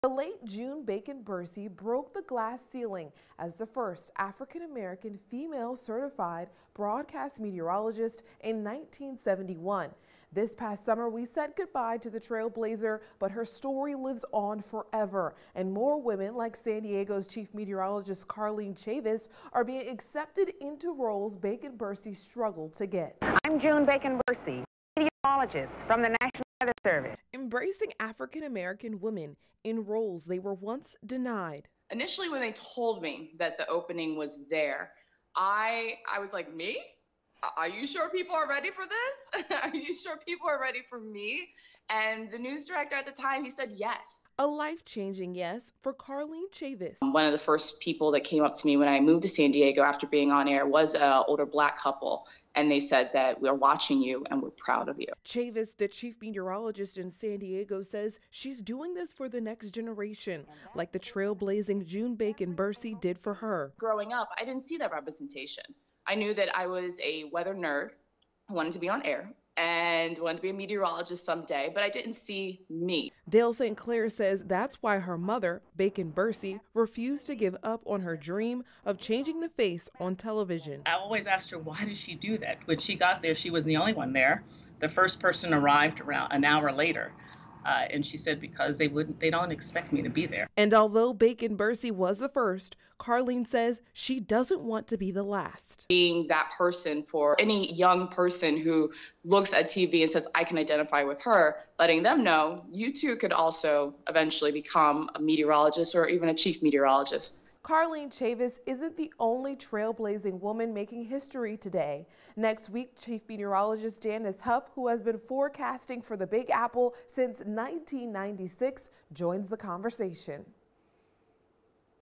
Black History Month: Remembering the First African-American Female Meteorologist
Meteorologist Monique Robinson speaks with CBS San Diego's Chief Karlene Chavis.